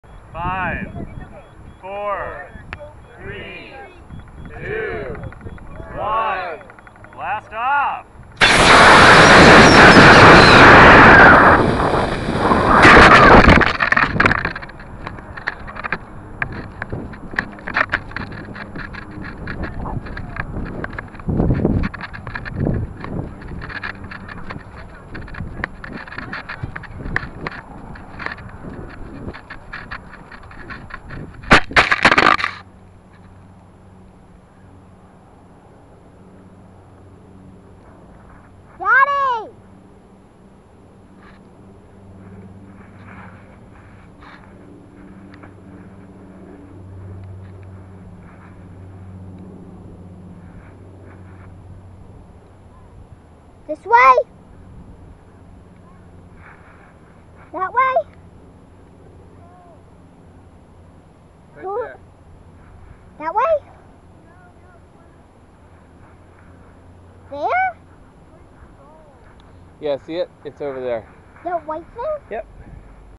Rocket Launch 2014 June - 2 of 6
Model rocket launch at 60 Acres. DATE ON VIDEO OVERLAY IS INCORRECT, natch. 808 keychain cams are great but I am always too rushed during launch days to change the date tag file.